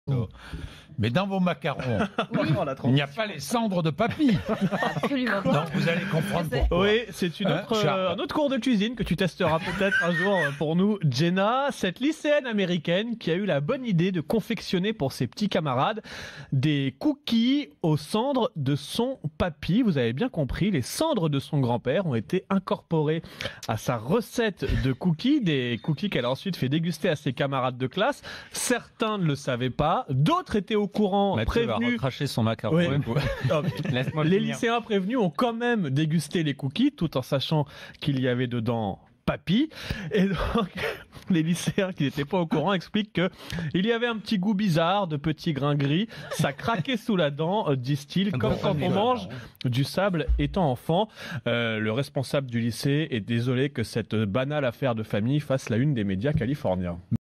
En Californie, les cendres de papy étaient dans les cookies
Une lycéenne américaine affirme avoir confectionné des cookies en incorporant les cendres de son grand-père à la recette avant de les faire déguster à des camarades de classe...